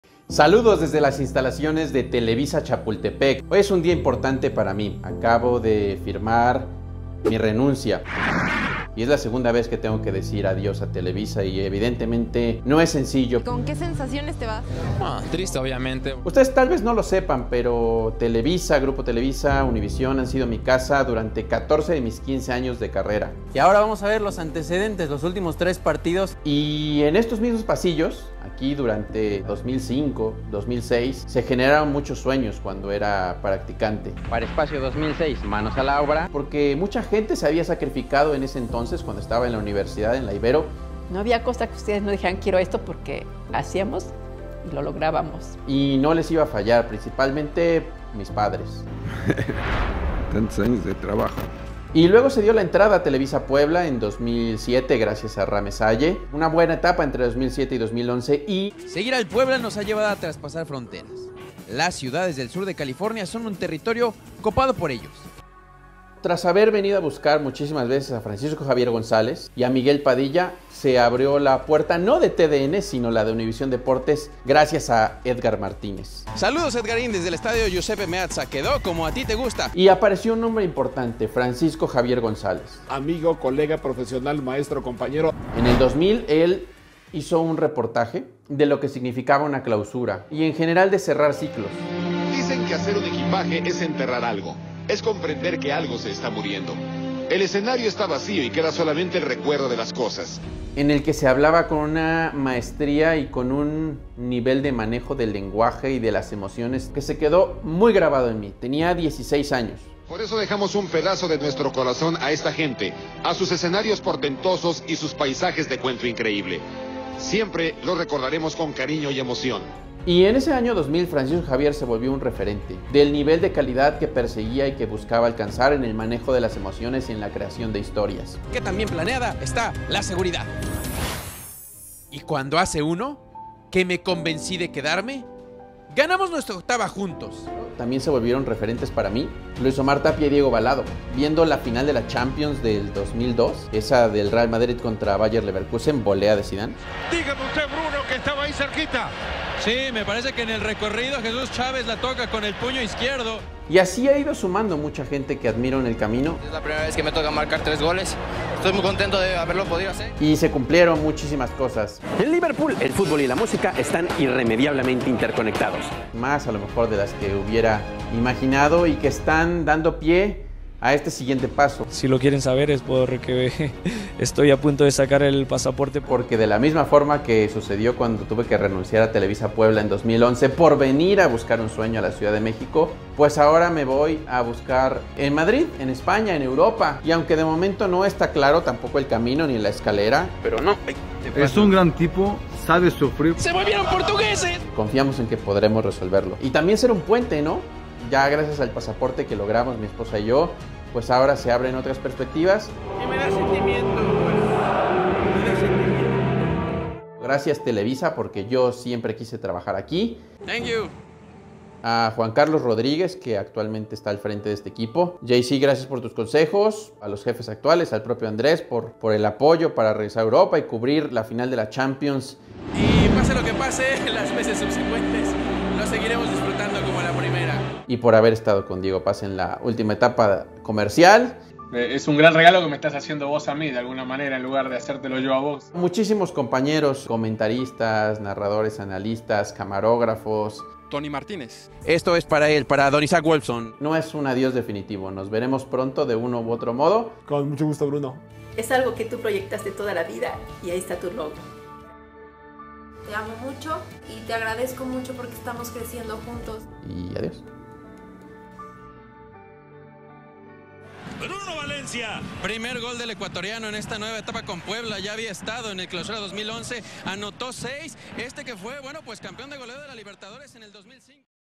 DEJO Univision México, ME VOY (vine) a EUROPA a construir mi/NUESTRO: El Sueño EUROPEO
En el verano de 2021, Bruno Valencia decidió renunciar a TelevisaUnivision la empresa mexicana - estadounidense en la que desarrolló la mayor parte de su carrera en medios de comunicación, para ir a buscar y crear algo que en ese momento no sabía que se llamaría "El Sueño EUROPEO"